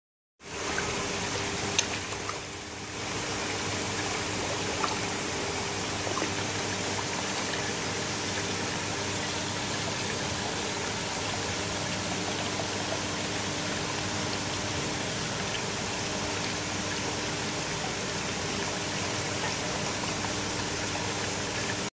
✅KOI.18*CÁ KOI NHẬT BẢN* Cá Koi đơn sắc và các dòng hi-ki-kinki utsuri của Nhật
Cá Koi Nhật nhập trại trong tháng 2/2016:
Yamabuki Ogon của Izumiya. Kin kiutsuri của Miyatora. Hi utsuri và Ki utsuri của Marusei. Chagoi của Matsue. Ginrin soragoi của Maruhiro
TUNGLAM GARDEN.,JSC